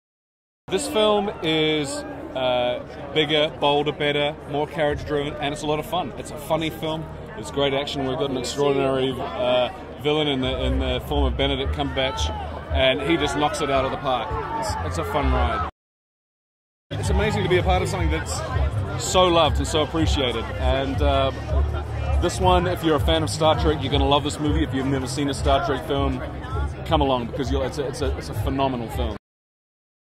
Star Trek Into Darkness London Premiere - Karl Urban Interview
The kiwi star talks about his character Bones

Plot: After the crew of the Enterprise find an unstoppable force of terror from within their own organization, Captain Kirk leads a manhunt to a war-zone world to capture a one man weapon of mass destruction.

Star Trek Into Darkness is in cinemas from the 9th of May